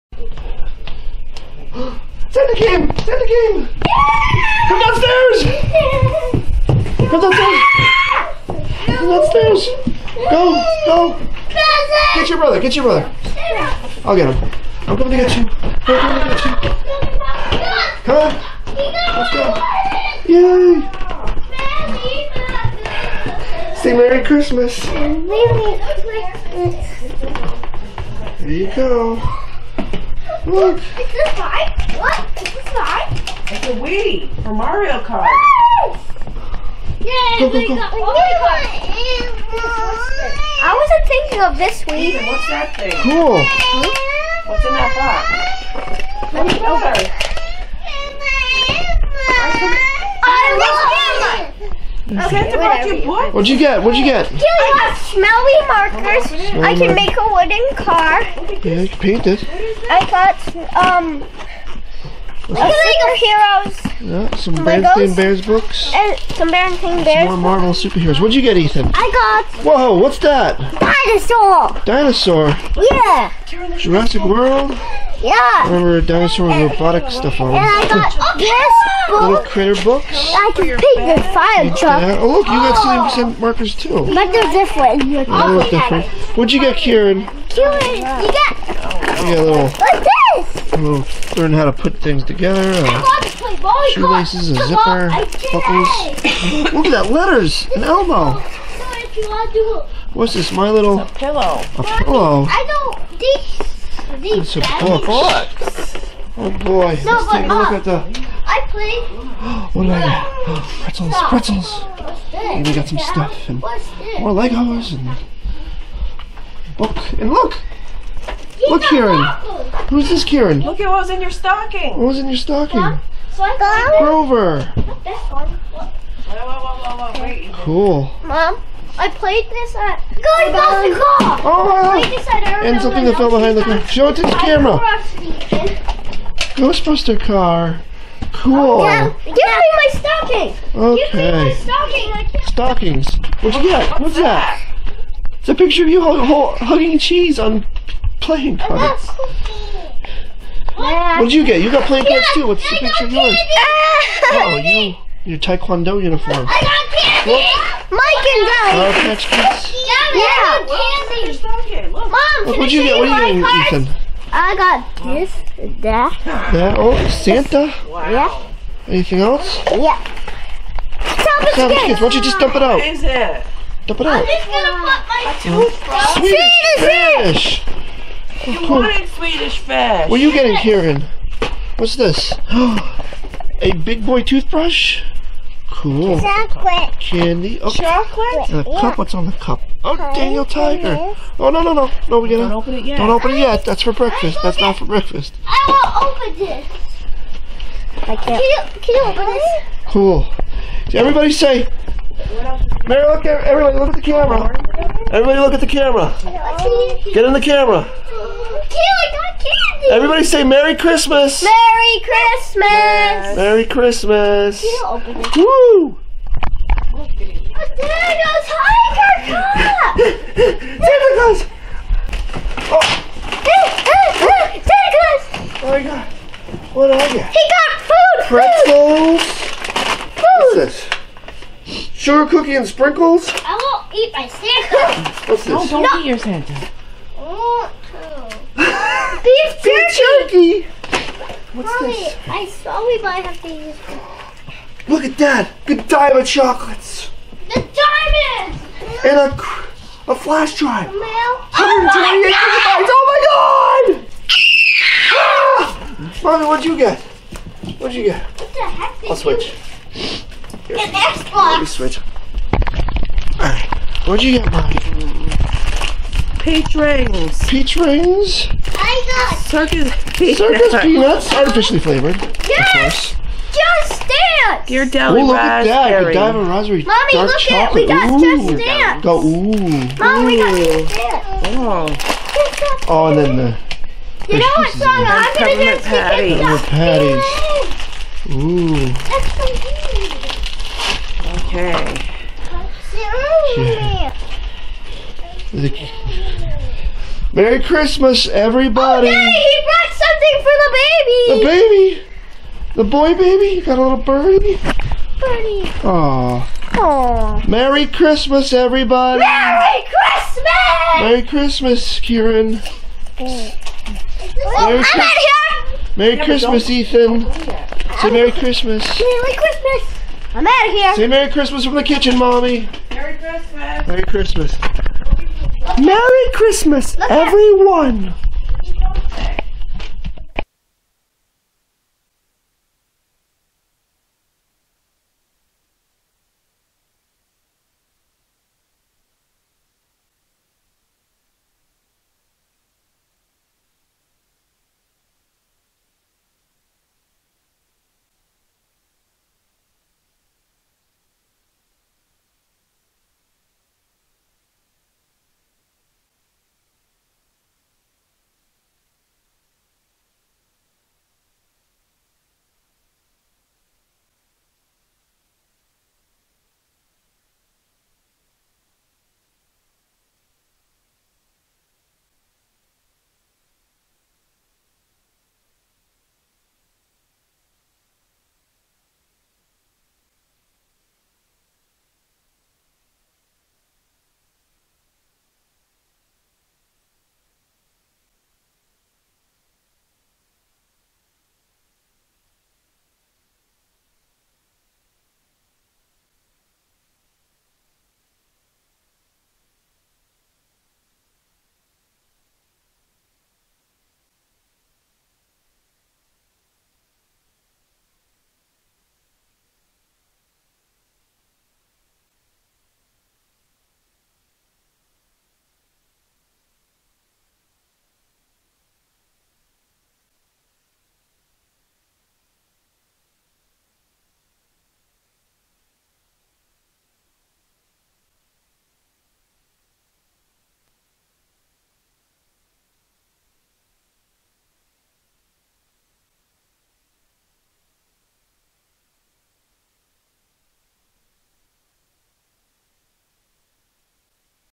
1,046 - Christmas Morning 2018 (Last 2 Minutes Silent)
It's X-Mas Morning 2018 with the boys! (Unfortunately the last two minutes of the video have no audio. So it's like an old timey video. lol) Enjoy! ... BE FRIENDS with JOEL RYAN on FACEBOOK